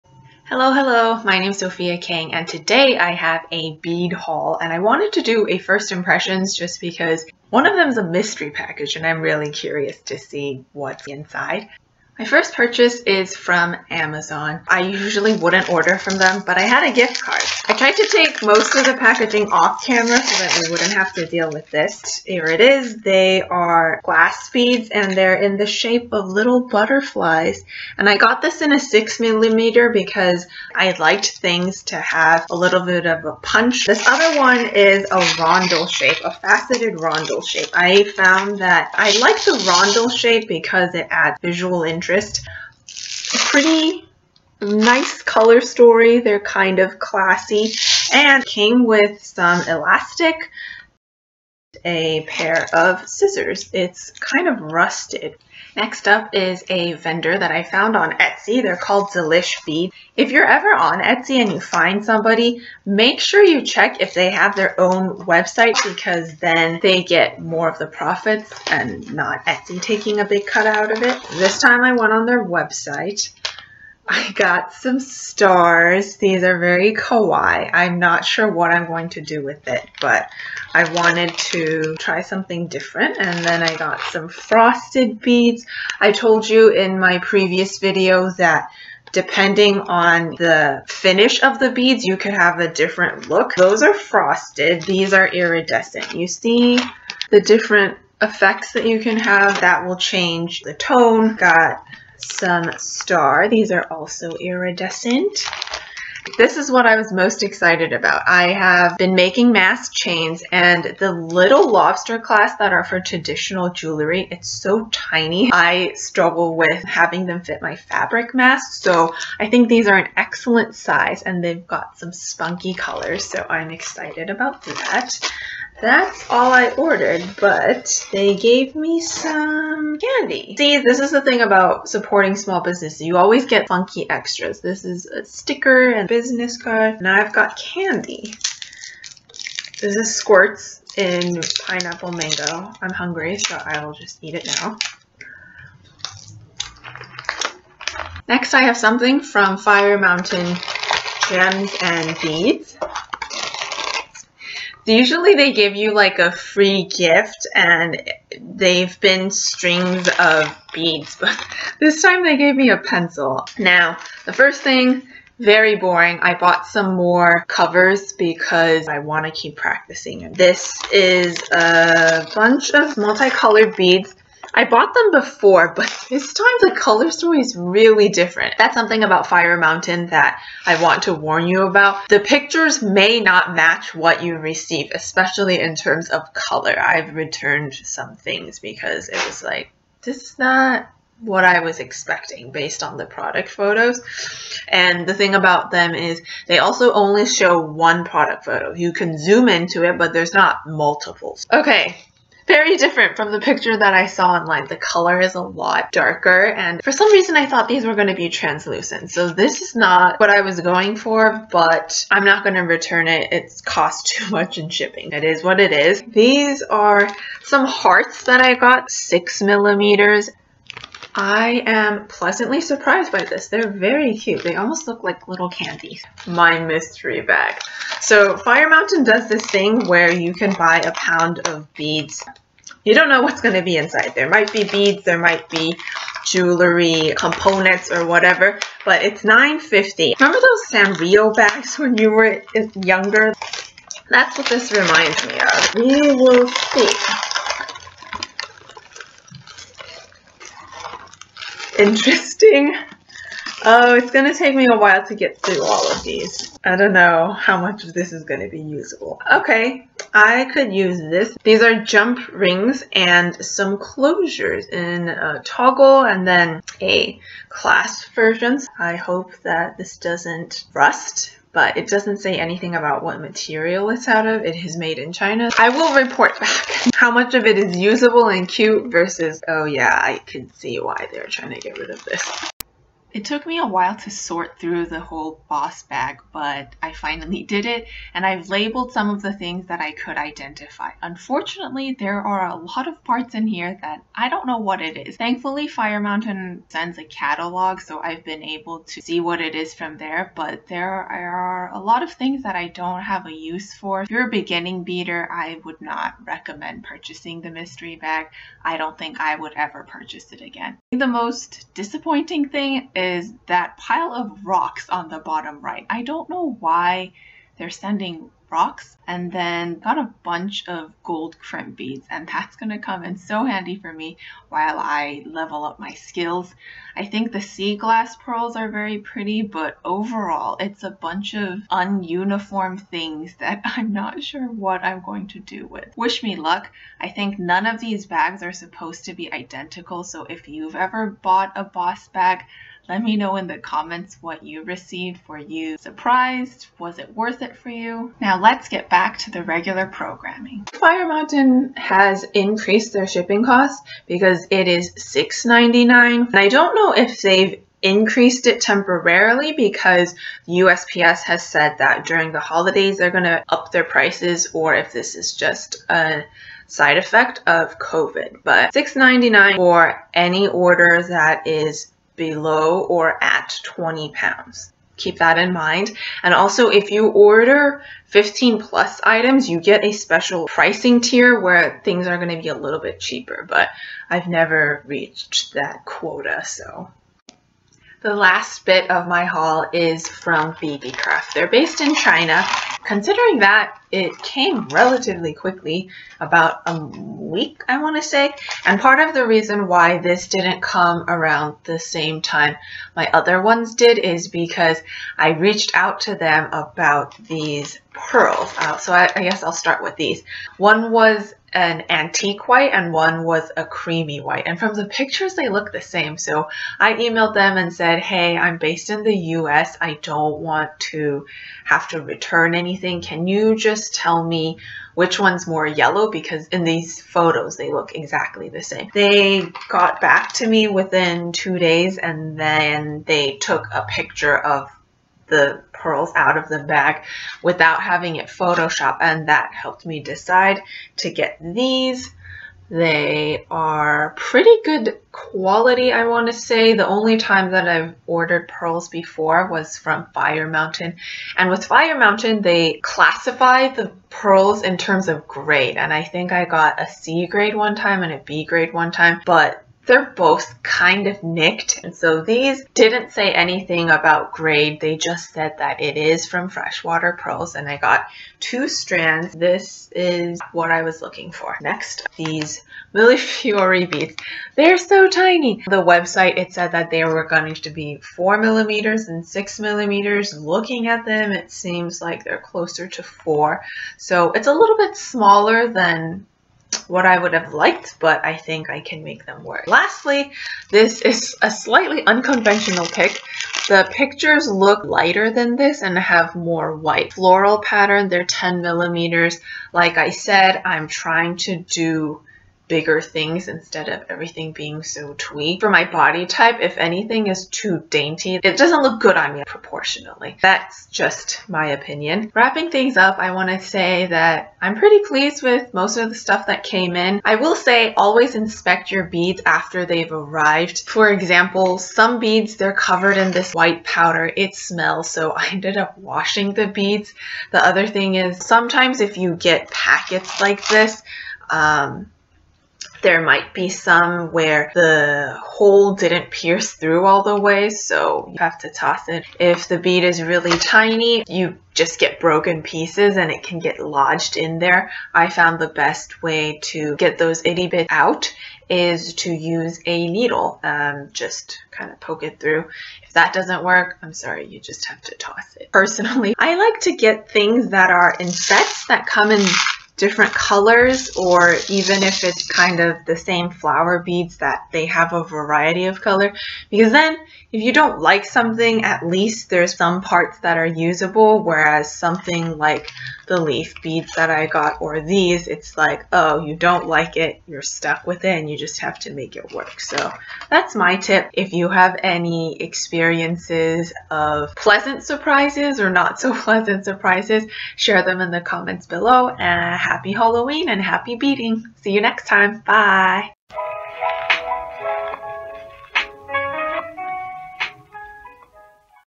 Bead Haul: First Impressions of Fire Mountain Mystery Bag and More
It's been quite a while since I ordered new beads online. I wanted to share a bit of what you can expect as well as show you the contents of my Fire Mountain Bosses Bead Bag, pearls from BeebeeCraft and more. You can't always trust the pictures you see online. I like to purchase sets or things that come in a set or variety of colors so that if it's not what I expected, I can salvage some of them. Do you know of any stores that sell good quality beads at a reasonable price? Would you purchase a Fire Mountain Bosses Bead Bag?